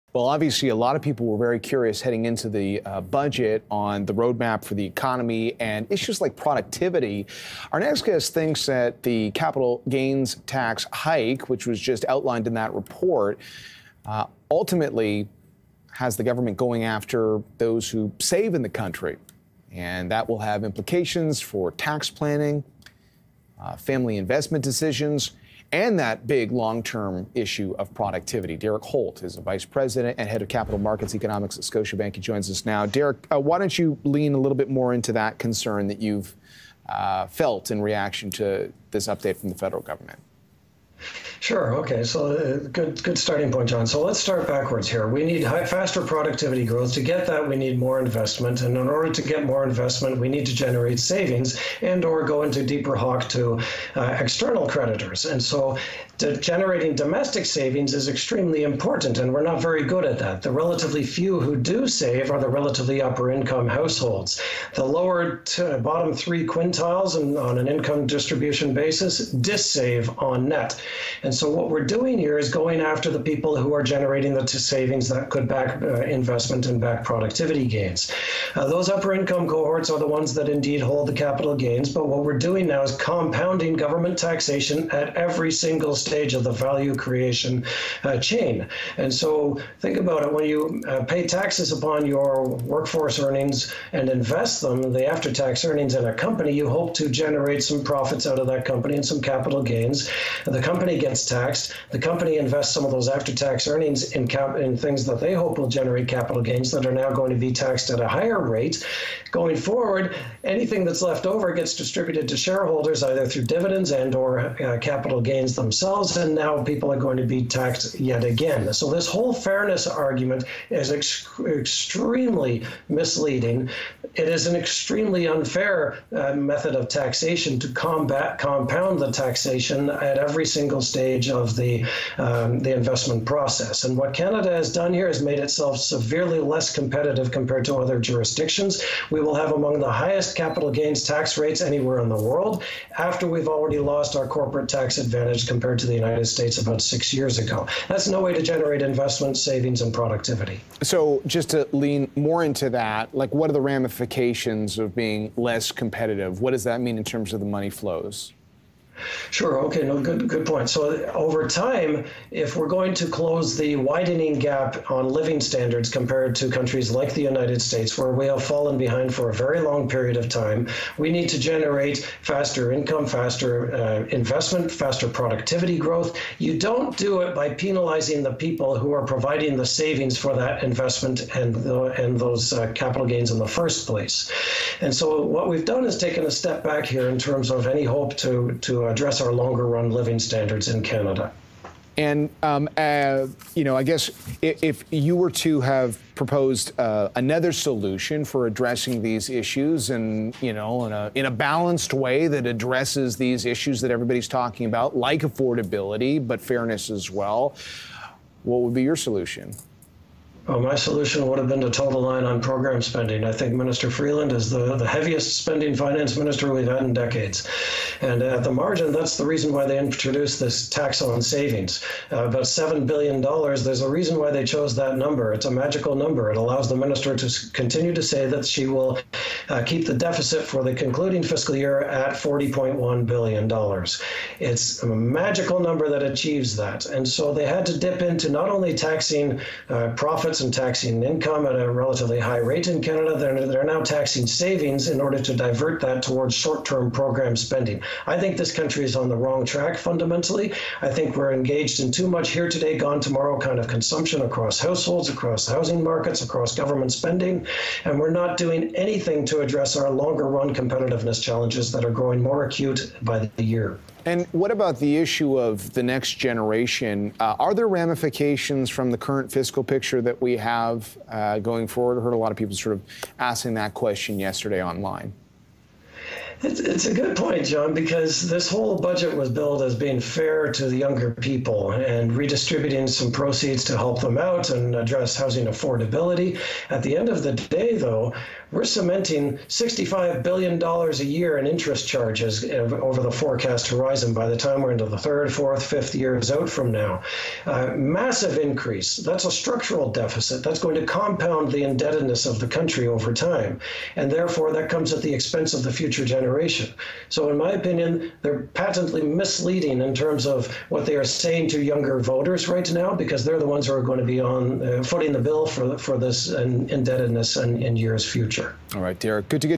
Federal government's capital gains implications for tax planning in investments and productivity
Derek Holt, vice president and head of capital markets economics, Scotiabank joins BNN Bloomberg to break down and preview the latest economic data, with a focus on the federal government’s 2024 budget, which revealed changes to how capital gains will be taxed.